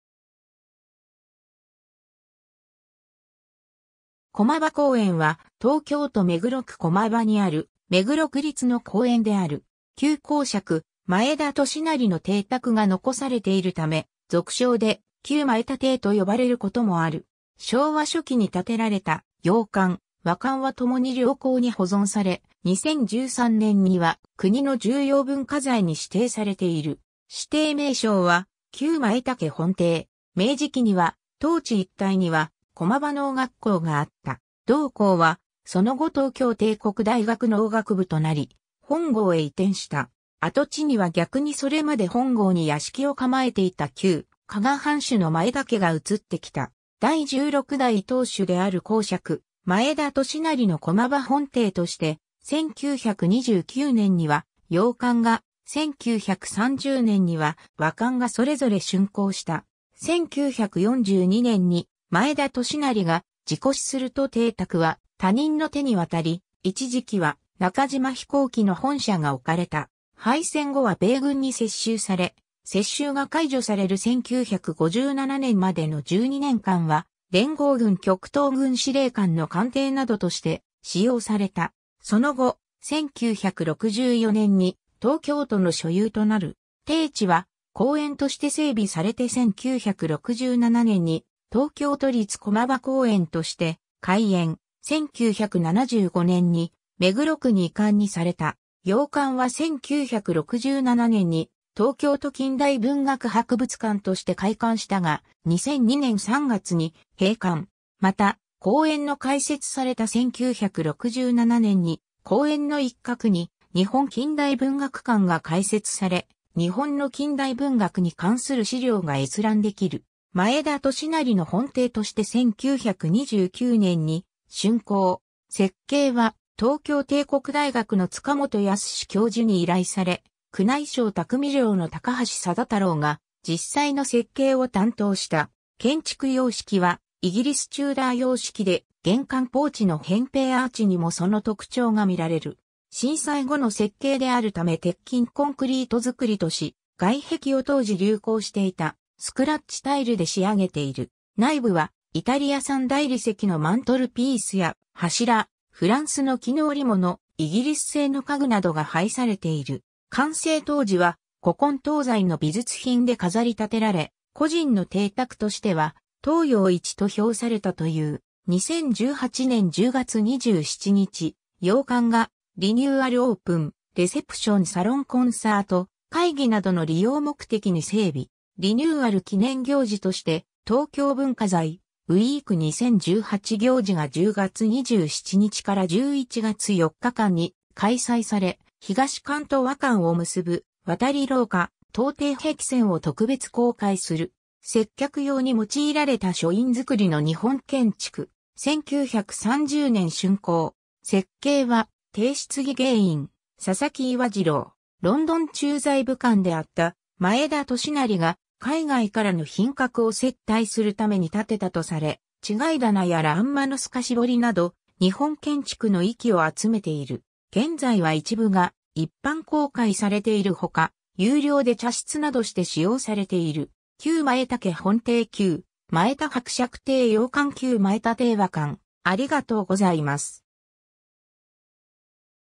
駒場公園 (目黒区)
駒場公園（こまばこうえん）は、東京都目黒区駒場にある目黒区立の公園である。
旧侯爵前田利為の邸宅が残されているため、俗称で旧前田邸と呼ばれることもある。
昭和初期に建てられた洋館・和館は共に良好に保存され、2013年には国の重要文化財に指定されている。
指定名称は「旧前田家本邸」。
 明治期には当地一帯には駒場農学校があった。
同校はその後東京帝国大学農学部となり、本郷（文京区弥生）へ移転した。
跡地には逆にそれまで本郷に屋敷を構えていた旧加賀藩主の前田家が移ってきた。
第16代当主である侯爵・前田利為の駒場本邸として、1929年（昭和4年）には洋館が、1930年（昭和5年）には和館がそれぞれ竣工した。
1942年（昭和17年）に前田利為が事故死すると邸宅は他人の手に渡り、一時期は中島飛行機の本社がおかれた。
 敗戦後は米軍に接収され、接収が解除される1957年（昭和32年）までの12年間は連合軍極東軍司令官の官邸などとして使用された。
 その後、1964年（昭和39年）に東京都の所有となる。
邸地は公園として整備されて1967年（昭和42年）に東京都立駒場公園として開園、1975年（昭和50年）に目黒区に移管にされた。
洋館は1967年に東京都近代文学博物館として開館したが、2002年（平成14年）3月に閉館。
また、公園の開設された1967年に公園の一角に日本近代文学館が開設され、日本の近代文学に関する資料が閲覧できる（有料）。
 前田利為の本邸として1929年（昭和4年）に竣工。
設計は東京帝国大学の塚本靖教授に依頼され、宮内省内匠寮の高橋貞太郎が実際の設計を担当した。
 建築様式はイギリス・チューダー様式で、玄関ポーチの扁平アーチにもその特徴が見られる。
震災後の設計であるため鉄筋コンクリート造とし、外壁を当時流行していたスクラッチタイルで仕上げている。
内部はイタリア産大理石のマントルピースや柱、フランスの絹織物、イギリス製の家具などが配されている。
完成当時は古今東西の美術品で飾り立てられ、個人の邸宅としては東洋一と評されたという。
 2018年（平成30年）10月27日、洋館がリニューアルオープン、レセプション・サロンコンサート・会議などの利用目的に整備。
リニューアル記念行事として、「東京文化財ウイーク2018」行事が10月27日-11月4日間に開催され、東館と和館を結ぶ渡り廊下・東庭壁泉を特別公開する。
 接客用に用いられた書院造の日本建築。
1930年（昭和5年）竣工。
設計は帝室技芸員・佐々木岩次郎。
ロンドン駐在武官であった前田利為が海外からの賓客を接待するために建てたとされ、違い棚や欄間の透し彫など日本建築の粋を集めている。
 現在は一部が一般公開されているほか、有料で茶室などして使用されている。
 旧前田家本邸 旧前田伯爵邸洋館 旧前田邸和館